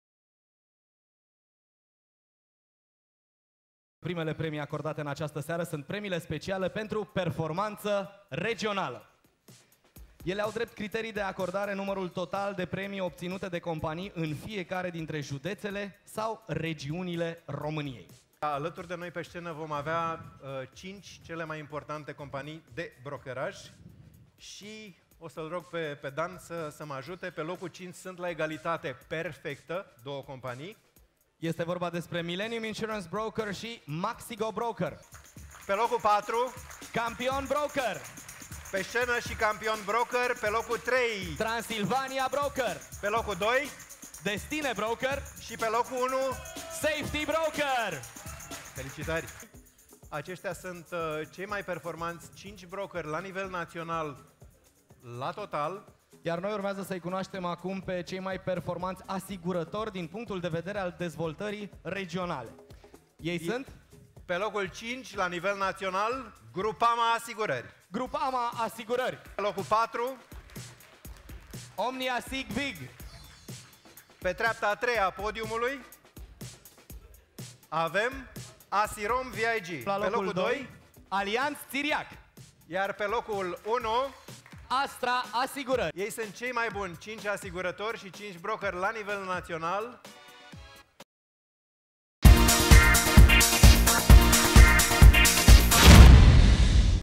PERFORMANTA REGIONALA Brokeri: SAFETY, DESTINE, TRANSILVANIA, CAMPION, MAXYGO, MILLENIUM
Gala Premiilor Pietei Asigurarilor Editia 2014. Video original aici